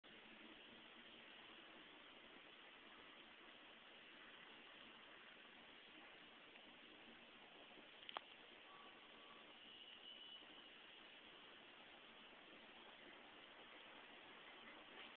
pretzel
I was bugging her while she was asleep. Heh.